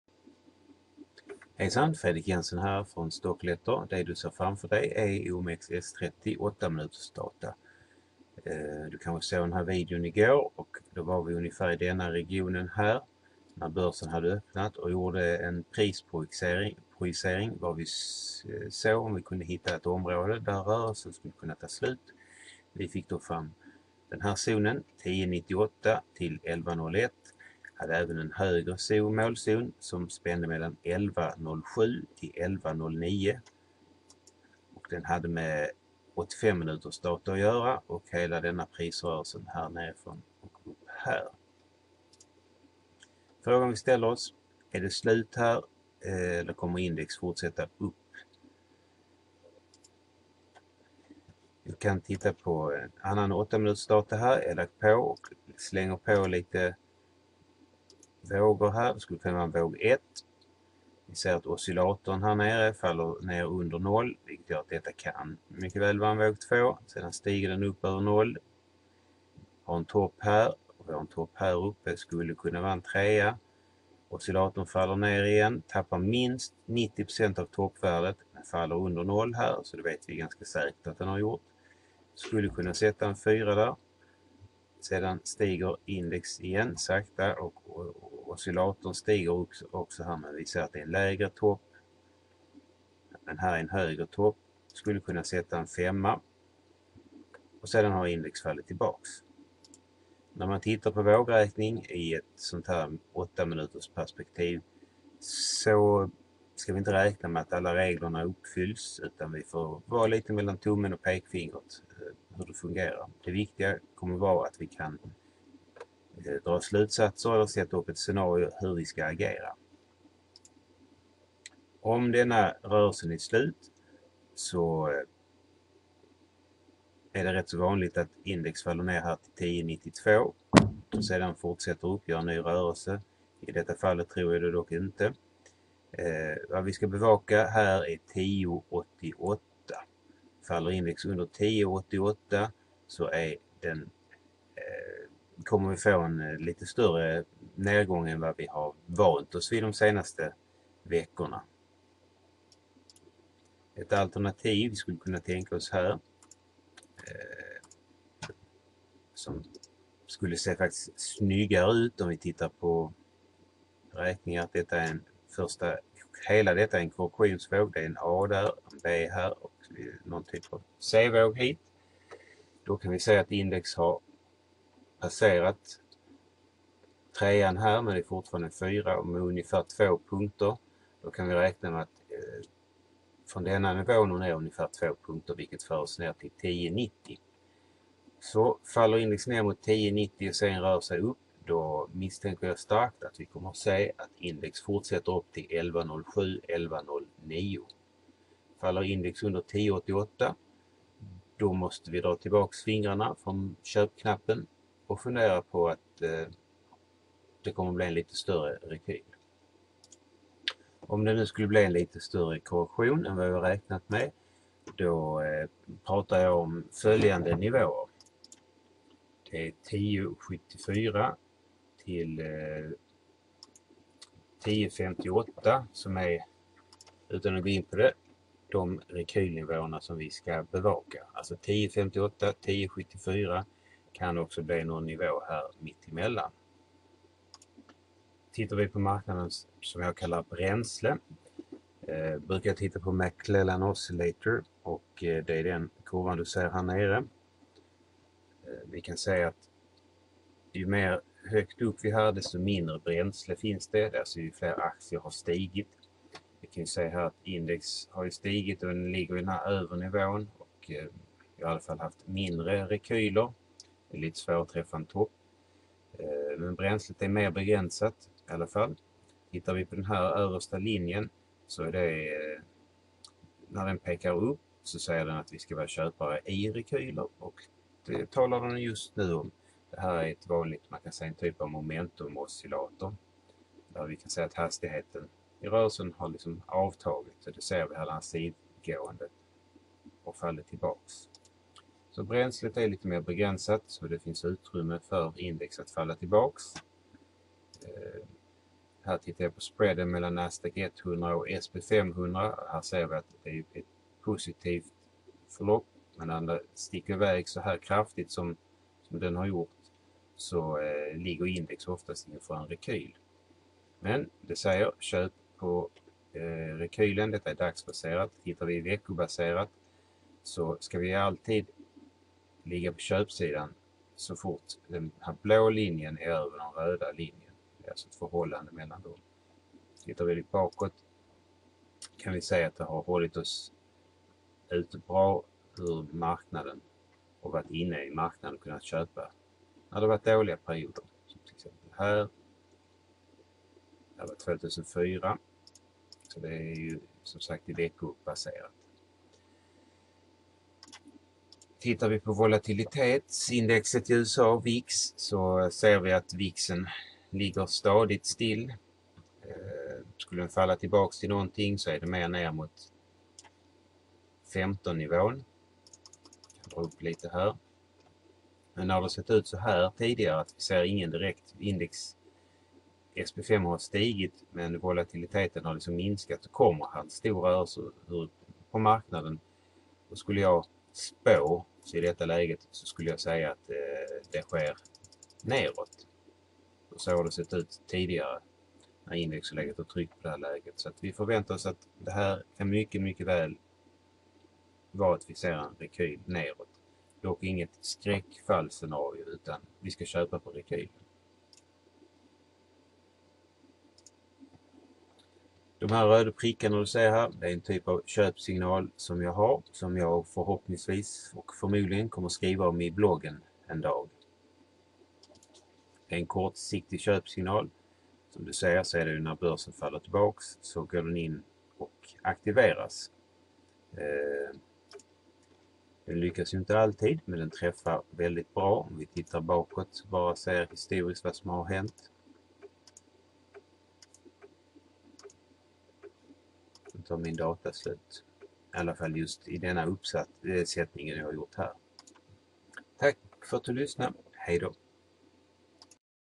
OMXS30 är det en topp vi har sett?
Fredrik Jensen från Stockletter visar OMXS30 som är ett index på Stockholmsbörsen. Du får prismål samt information om vart stöd och motstånd finns. Teknisk analys med praktisk Elliott Wave räkning visas. Men du ser även McCLellan Oscillator, VIX och andra teknisk analys metoder